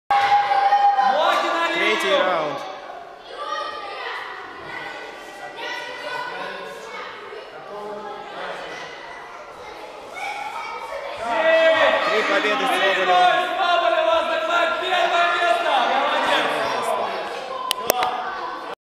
финал (3:0) Карандаши Соболева & Подымалова [Вармреслинг 2017]
Открытый турнир по силовой игре "Вармреслинг" на кубок Центра спорта Петроградского района Санкт-Петербурга.
Турнир проходил 21 декабря 2017 года, по адресу: Санкт-Петербург, Петроградский р-н., метро Горьковская, ул.Сытнинская 5/7, ГБОУ СОШ 91

С уважением,
руководитель СпортАктива